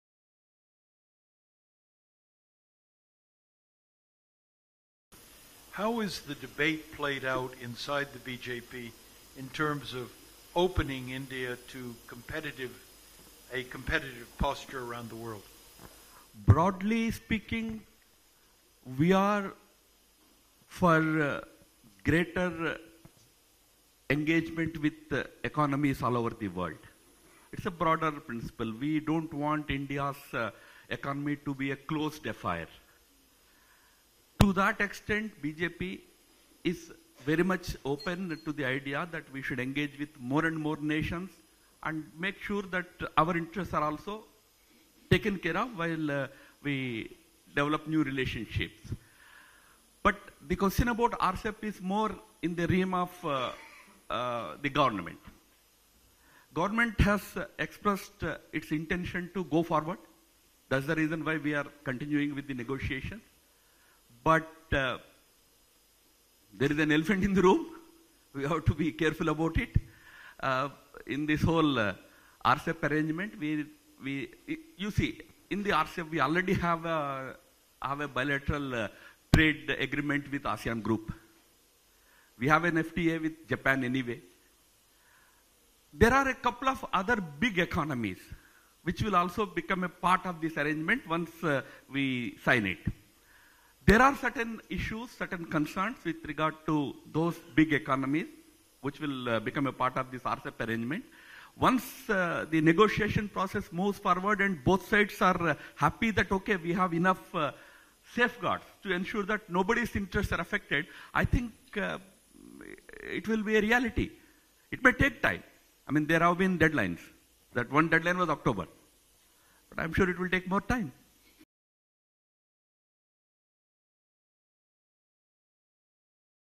BJP open to idea of India’s engagement with more countries, developing new relationships: Ram Madhav
BJP National General Secretary Ram Madhav said that his party is open to the idea that India should engage with more and more nations while taking care of own interests. Speaking at US-India Strategic Partnership Forum, Madhav said: “BJP is open to the idea that we should engage with more and more nations and make sure our interests are also taken care of while we develop new relationships.”